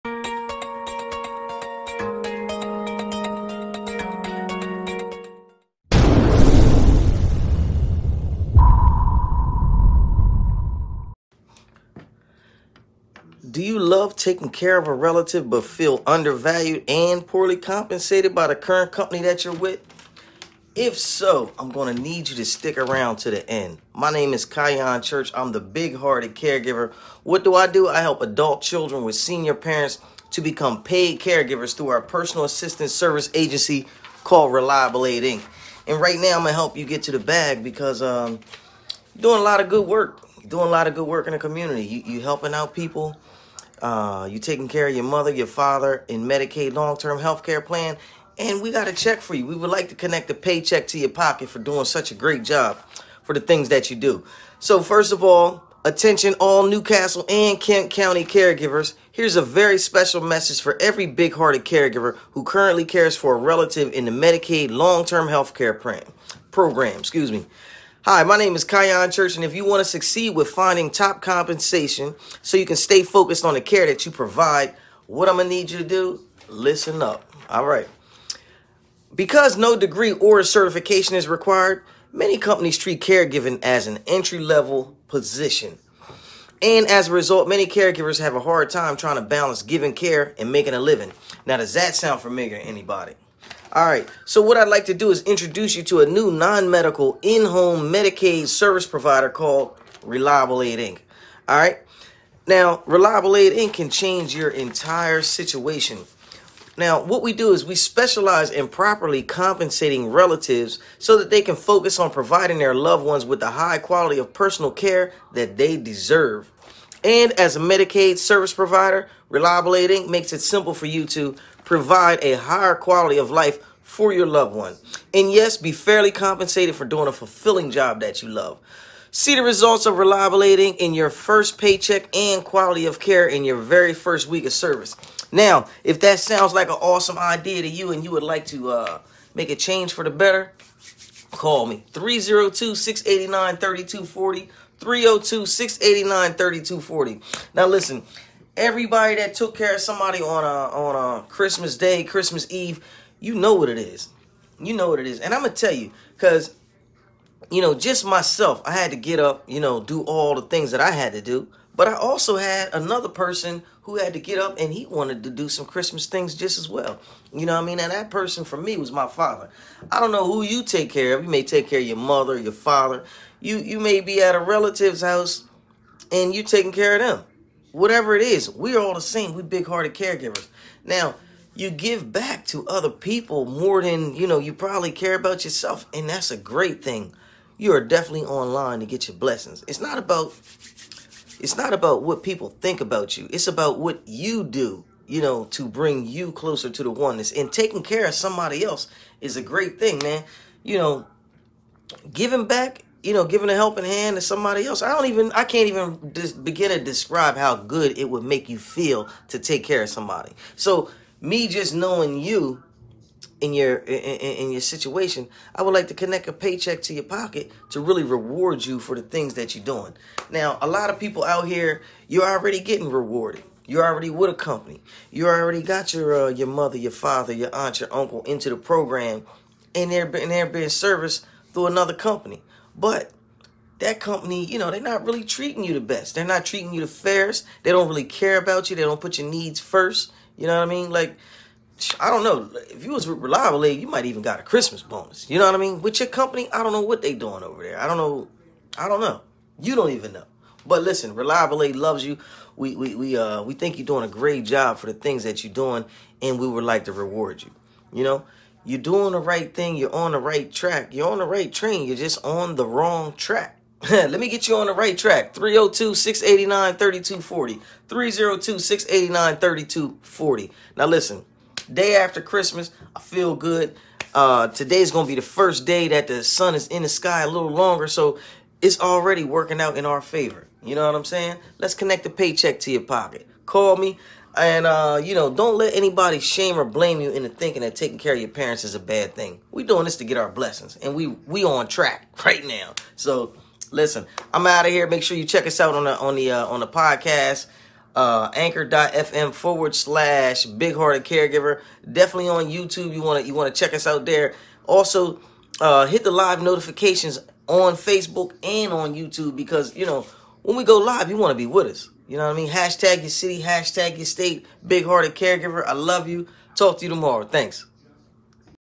Would You Like “TOP” Compensation?
Do you love taking care of a relative but feel undervalued  & poorly compensated by the current company your with?
Well New castle & kent county Caregivers
Listen-up!
Here's a very special message for every Big-Hearted Caregiver who currently cares for a relative in the Medicaid long term health care program.
Hi, my name is Khyon Church
and if you want to succeed with top compensation, so you can stay
focused on the care you provide, listen up!
Because no degree or certification is required, many companies treat caregiving as an entry level position.
As a result, many caregivers have a hard time trying to balance caregiving& making a living.
Sound familiar?
I'd like to introduce you the Best, in-home Medicaid Service Provider called Reliable Aid Inc.
We specialize in properly compensating relatives so they can focus on providing their loved ones with the high quality personal care they deserve!
As a Medicaid Service Provider, Reliable Aid Inc" makes it simple for you to:
Provide a higher quality of life for a loved one..
and yes! Be fairly compensated for doing a fulfilling job you love!
See the results of Reliable Aid Inc
in both your paycheck, and quality of care in the very first week of service.
So click the button below to complete the form and get started.

Reliable Caregiver